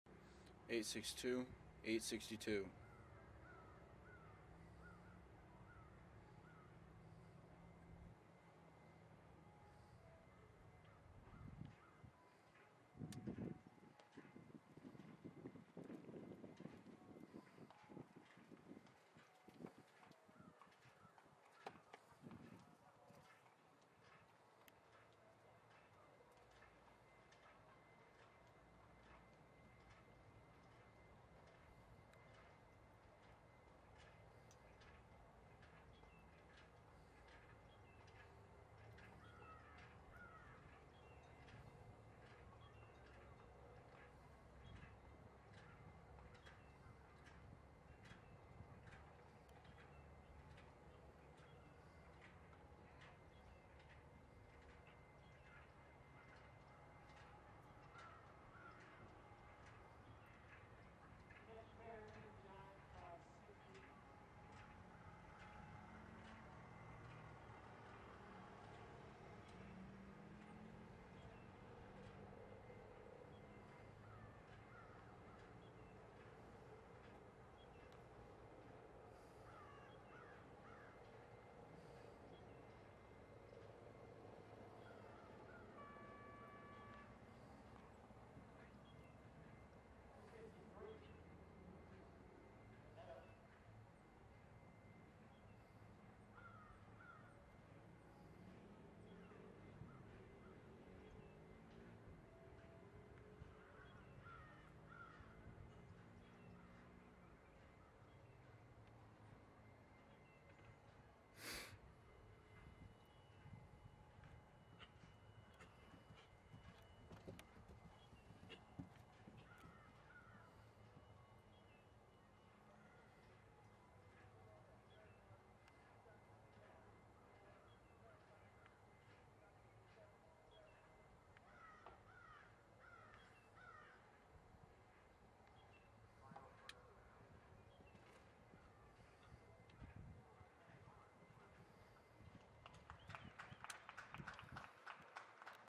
Video of Cashmero ridden by Morgan Ward from ShowNet!
Cashmero ridden by Morgan Ward at Ocala Winter Festival - V .  This round is from class 47, $250 Green Hunter 3'3 O/F and finished 4 of 23.